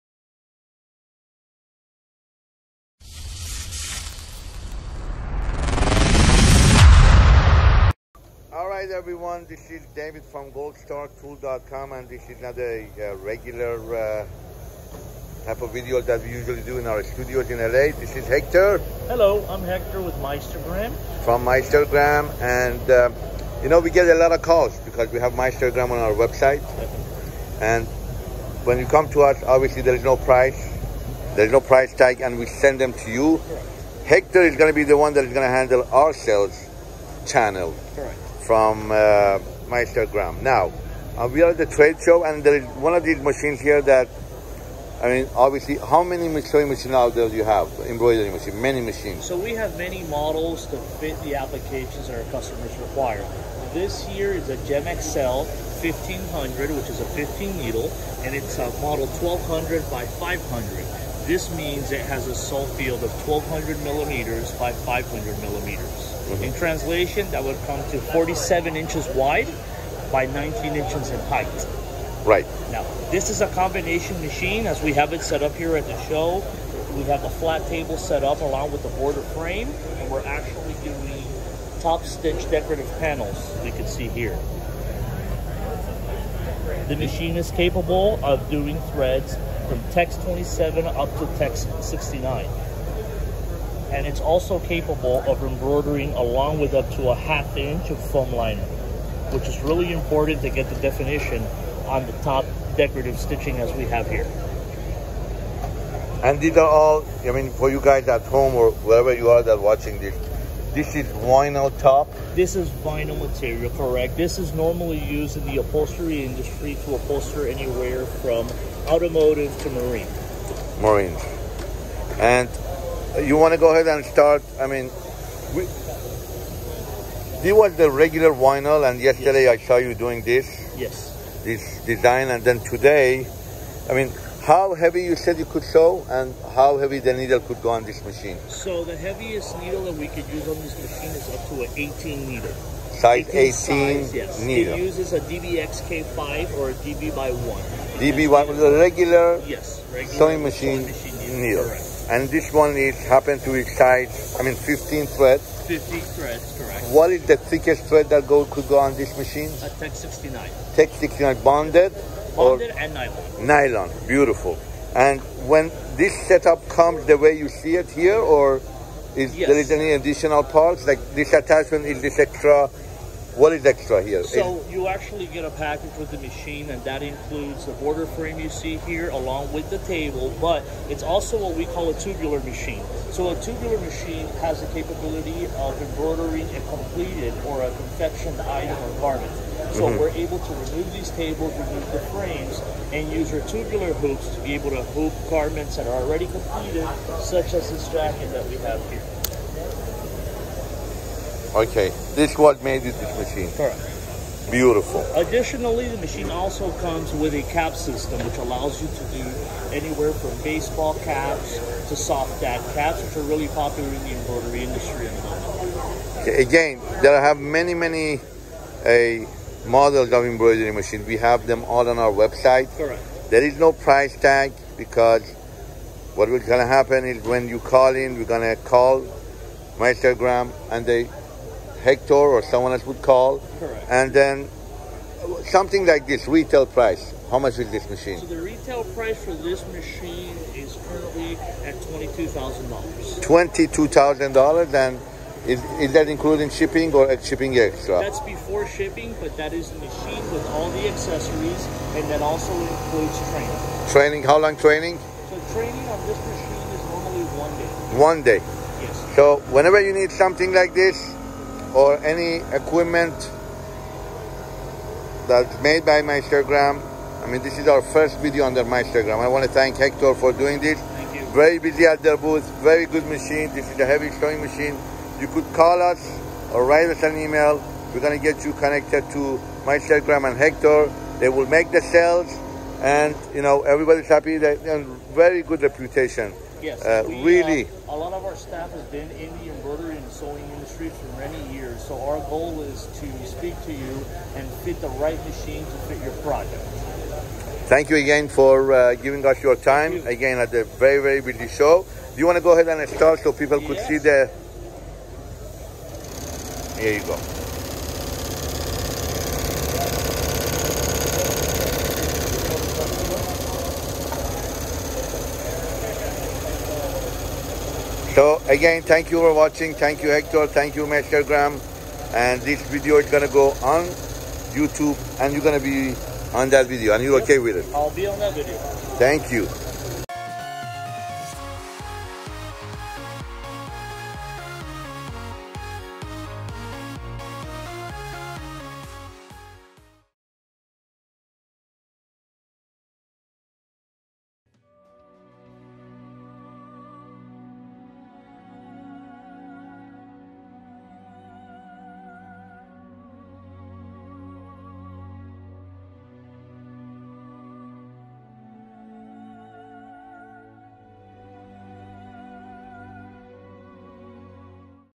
Trade Show - Meistergram Embroidery Machines - Goldstartool.com
In today's video, Hector from Meistergram (GoldstarTool's rep) talks to David about the features of their GEM Series,  materials that can be used with the machines, and more!